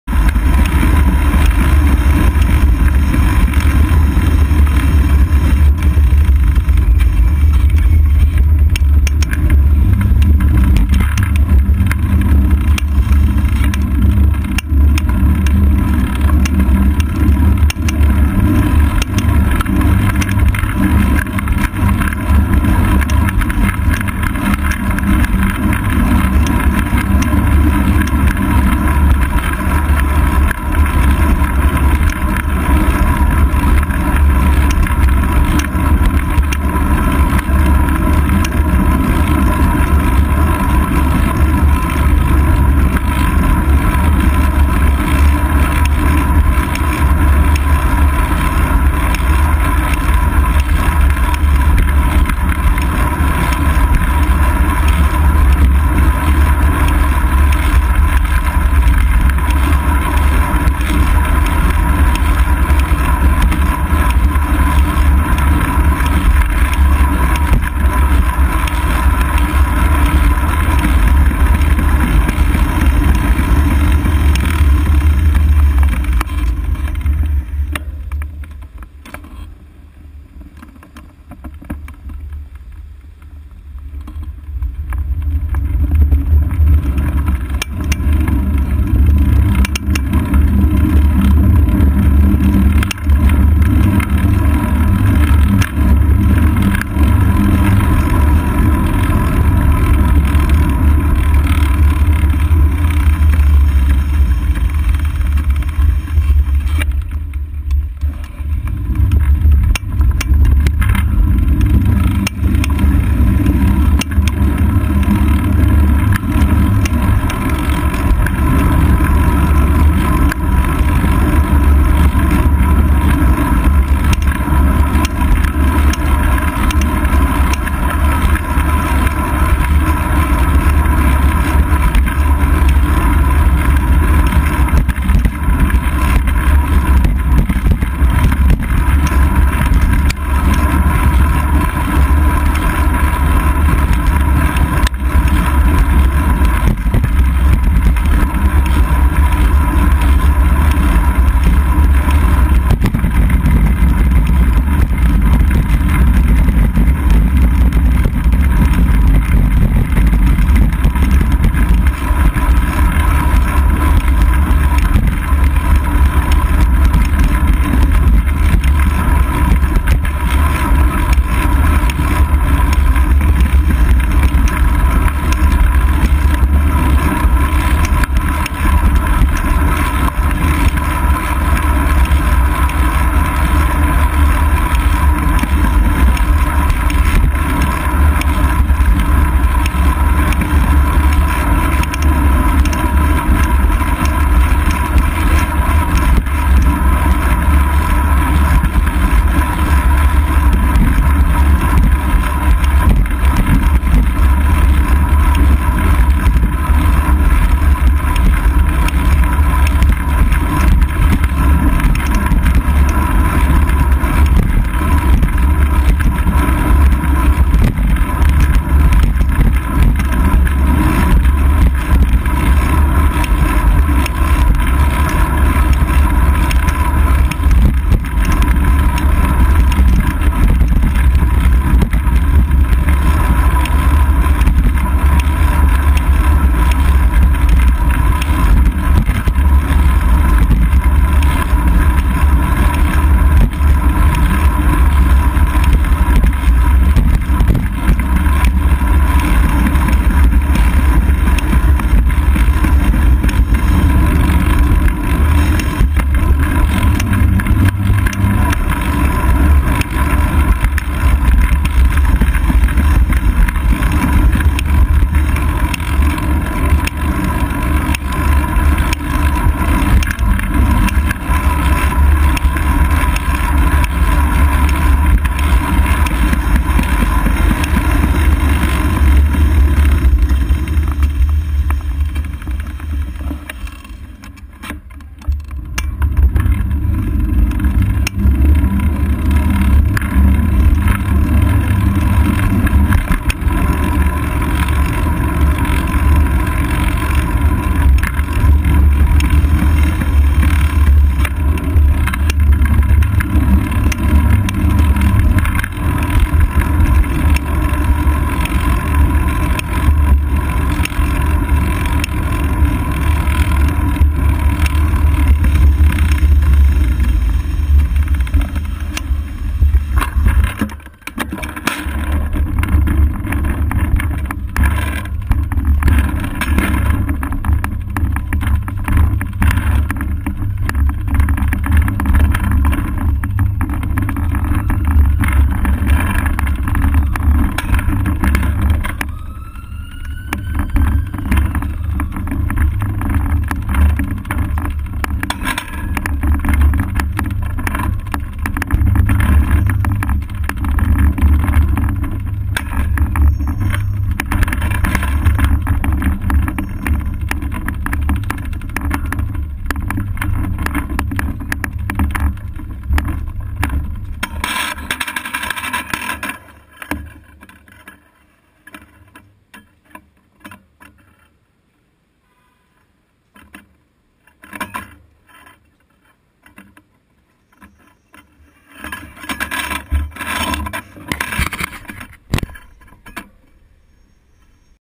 Tuesday evening commute - Part 2 - rear camera - 20131001 - GP010002
Almost hit on the way home due to my carelessness on 8th avenue.  rear camera has the scary view

Biking through the streets of NYC on the way home from  work on the evening of October 01, 2013.  This is the rear facing raw video footage shot from a seat post mounted GoPro Hero 2 camera, recorded at 1280x720, 60 fps, & 170º FOV wide angle.
Recorded 20131001 - Part 2 - GP010002